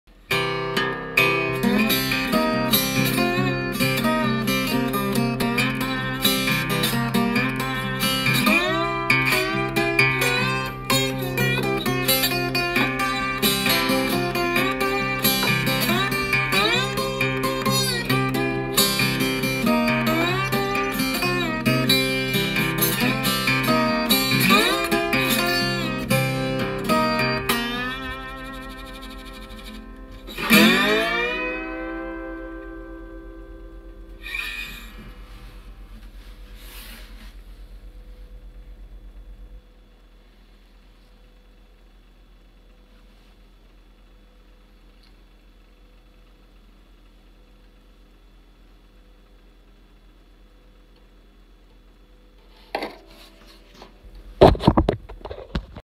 Oscar Schmidt made First Hawaiian Conservatory Of Music
Blind Willie Johnsonが写真の中持っているギターのブリッジが珍しい髭ブリッジバージョン。リペアの形跡が無いのでオリジナルと思われる。ラップギターなのでナットとサドルが高めの物になっていて指板には音が分かり易いように説明の紙が貼ってある。実はこのギター昔々レッスンの契約をするとタダでもらえたというもの。そんなギターホントにBWJが使ってたのかな？ニワカに信じ難いが。このギター買う時に指板の紙は取ってナットもサドルも低くしてフィンガリングもできるようにしようと思ってたけど剥がすのもったいないのでラップギターを普通に抱えてビデオを撮ってみた。それにしてもこんなお肌カサカサなギターは初めてだ。